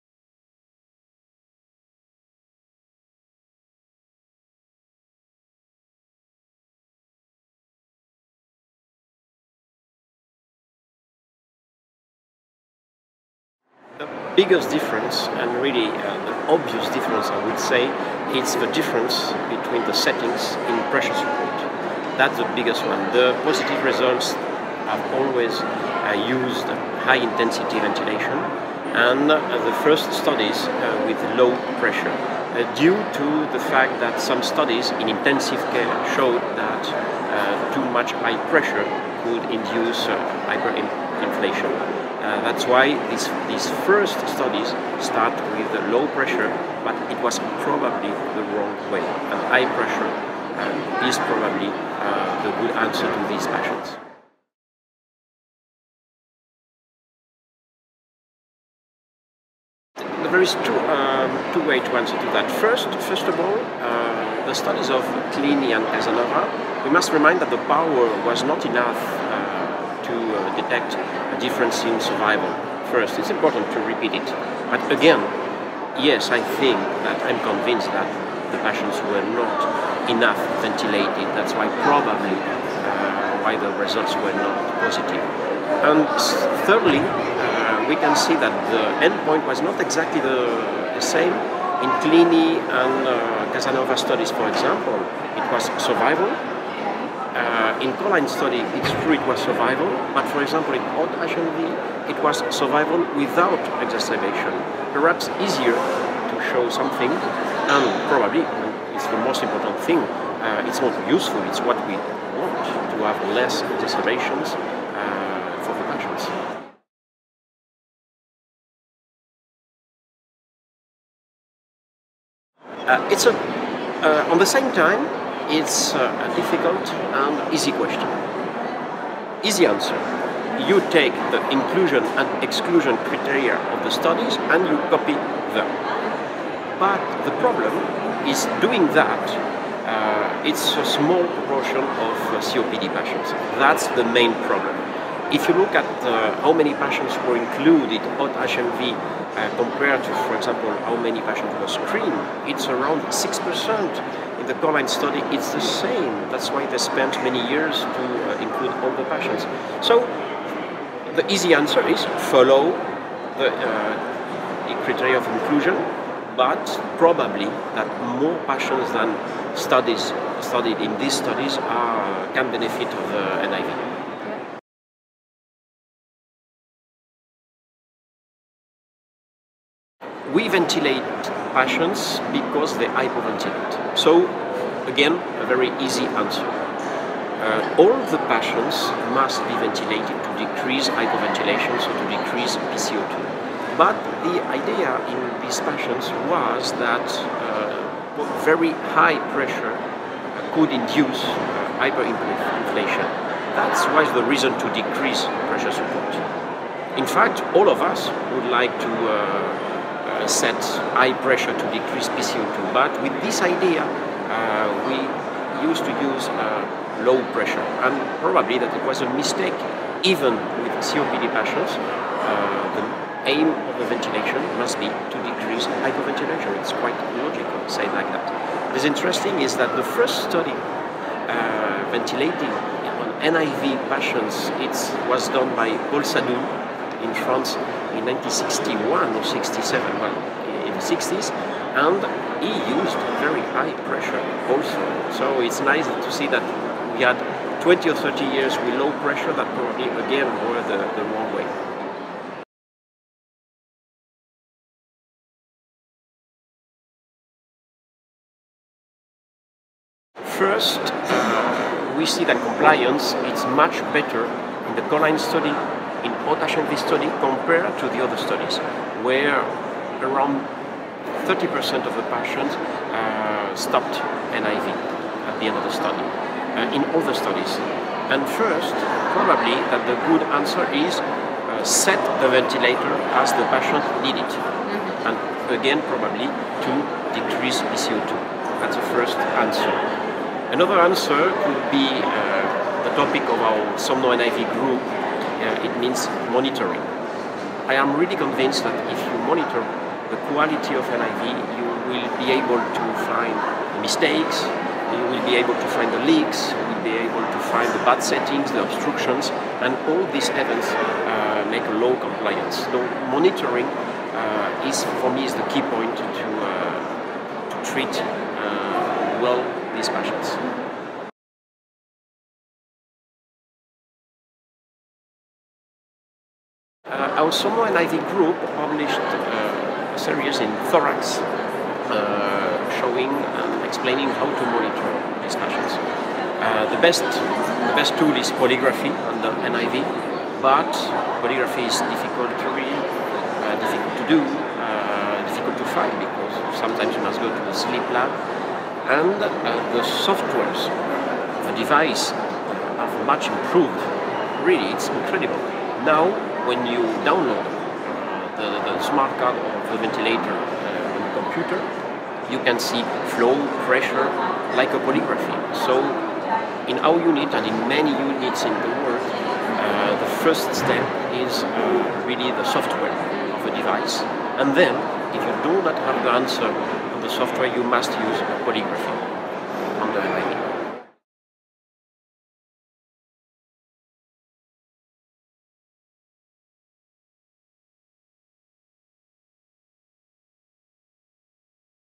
How to reconcile with high intensity NIV in home COPD patients -Pr Jesus Gonzalez-Bermejo
Interview, ERS 2018 – Pr Jesus Gonzalez-Bermejo, Pulmonologist, Respiratory rehabilitation unit - Hôpital Pitié Salpêtrière, Paris, France

“The easy answer is “follow the criteria for inclusion” but it is likely that more patients than were included in these studies could benefit from NIV.”